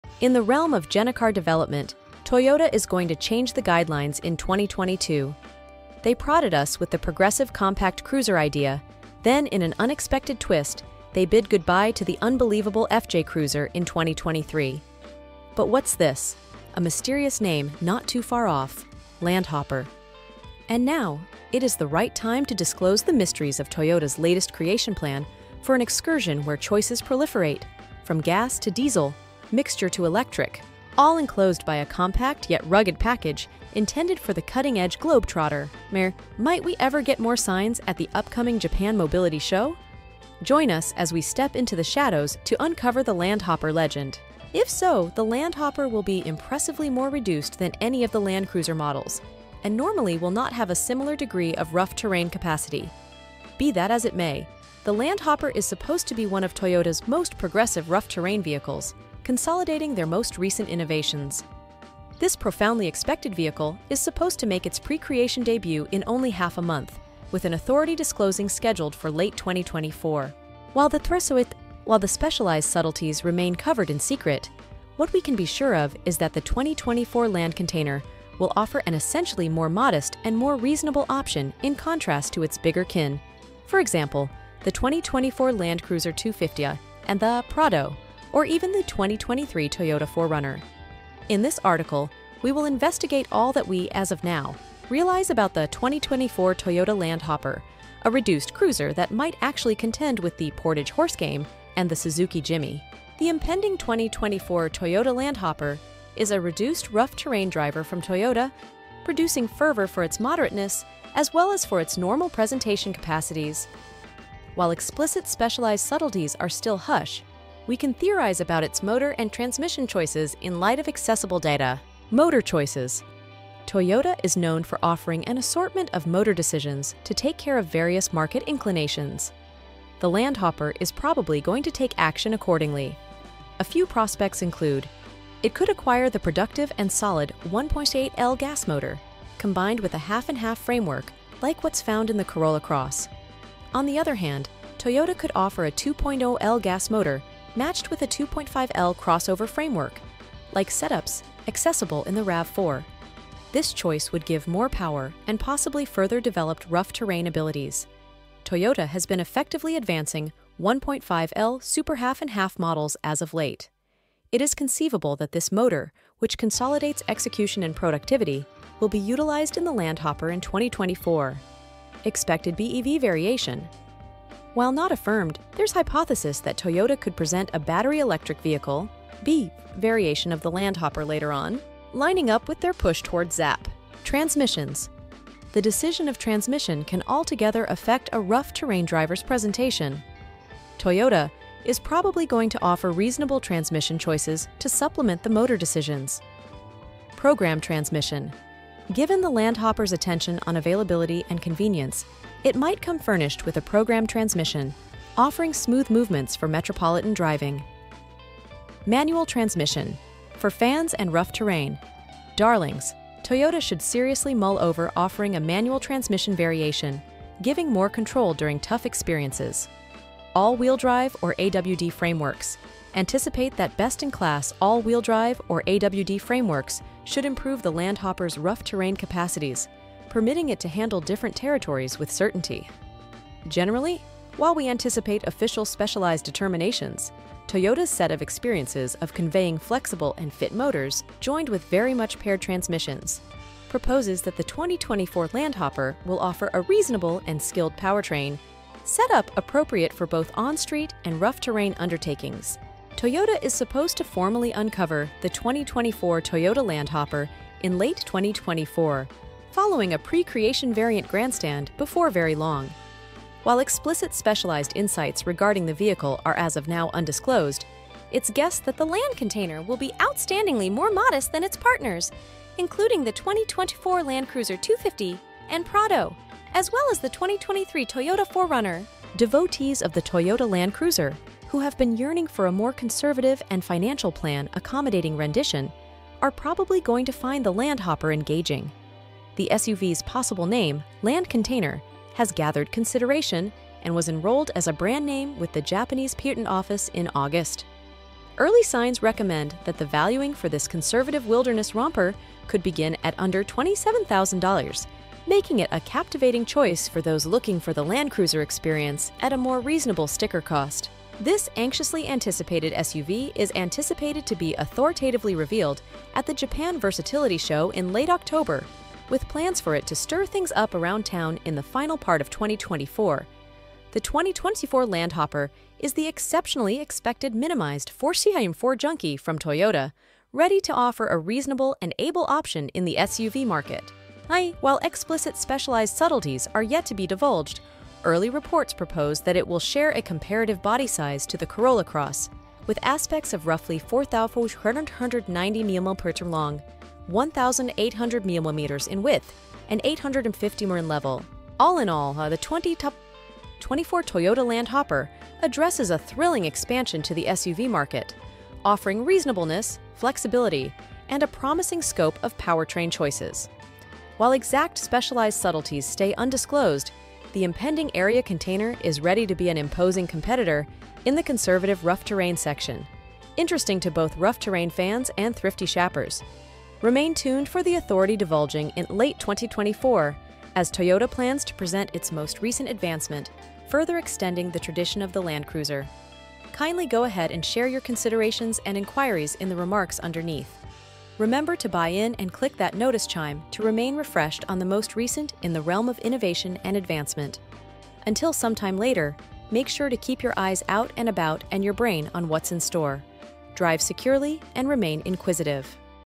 2025 Toyota LAND HOPPER MINI - BABY Land Cruiser!
In this exhilarating video, we invite you on an adventure to explore Toyota's latest addition to its lineup – the Land Hopper Mini. Get ready for an immersive journey as we unveil the exceptional features and capabilities of this compact yet robust off-road vehicle, designed to redefine your outdoor experience.

Uncover the Land Hopper Mini's resilient design, meticulously crafted to effortlessly navigate the most challenging terrains. From its impressive ground clearance to its durable suspension system, this mini SUV is engineered to conquer any trail with confidence.

We'll also dive into the remarkable performance of the 2025 Land Hopper Mini, delving into its potent engine options, exceptional traction control, and advanced off-road driving modes. Whether you're a seasoned off-road enthusiast or a newcomer to the adventurous world, this vehicle caters to a diverse range of preferences.

Step inside to discover a comfortable and technologically advanced interior that seamlessly combines rugged capability with modern convenience. We'll showcase the spacious cabin, user-friendly infotainment system, and safety features that ensure a secure and enjoyable journey wherever your exploration takes you.

Join us as we put the Land Hopper Mini through its paces on various terrains, from rocky trails to muddy paths. Witness the incredible off-road action and experience firsthand the Mini's outstanding performance. Whether you're an adventure seeker in search of the ideal vehicle to fuel your wanderlust or simply intrigued by the future of off-roading, hit that play button and immerse yourself in the world of the 2025 Toyota Land Hopper Mini!

02: 02 Motor choices
03: 04 Transmissions
04: 39 Price